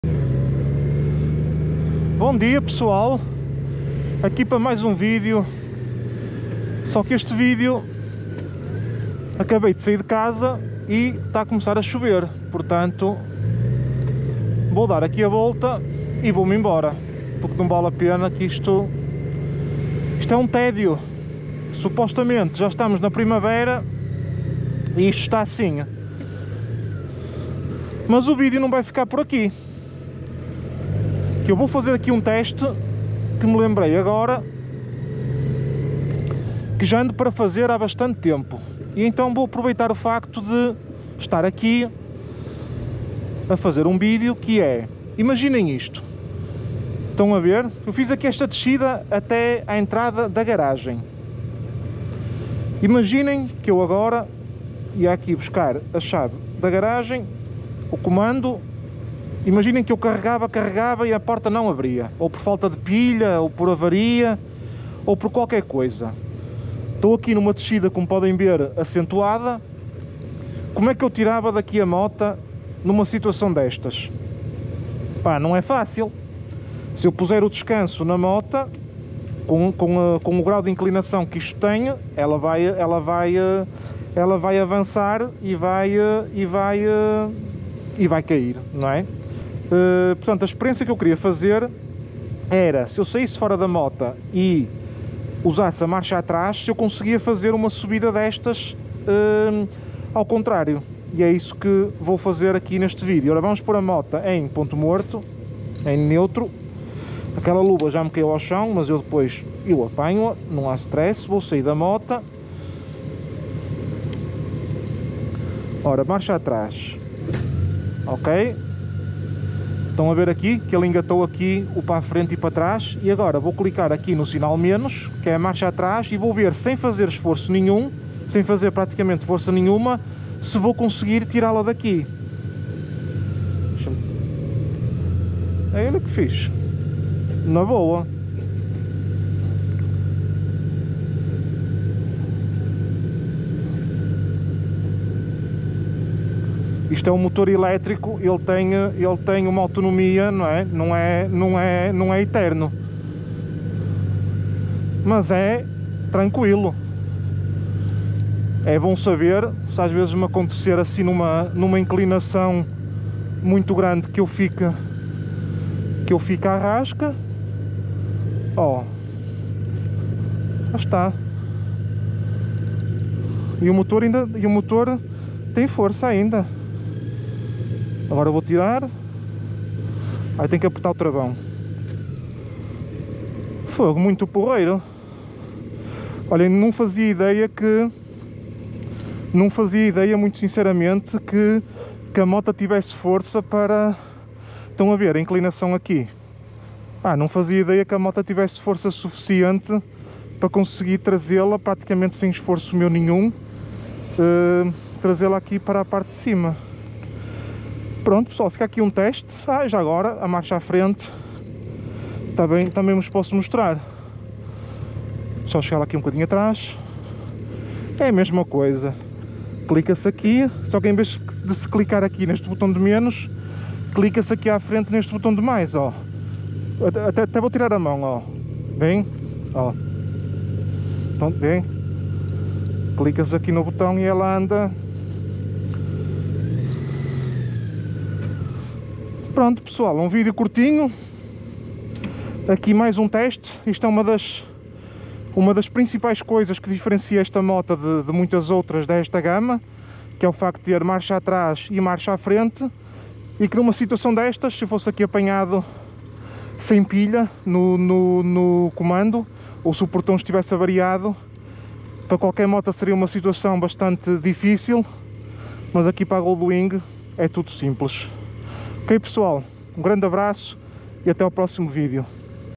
Testar a MARCHA ATRÁS da Honda Gold Wing numa DESCIDA
Vê como coloco a Honda Gold Wing no derradeiro teste de marcha atrás numa descida.

Vê como esta poderosa máquina lida com o desafio com facilidade e precisão. Quer sejas um entusiasta da Gold Wing ou apenas um fã de feitos impressionantes com motos, este vídeo certamente vai-te impressionar. Não percas o vídeo - Carrega já no Play!

Subscreve o canal para mais conteúdos interessantes sobre motos e ativa o sino de notificação para nunca perderes uma atualização.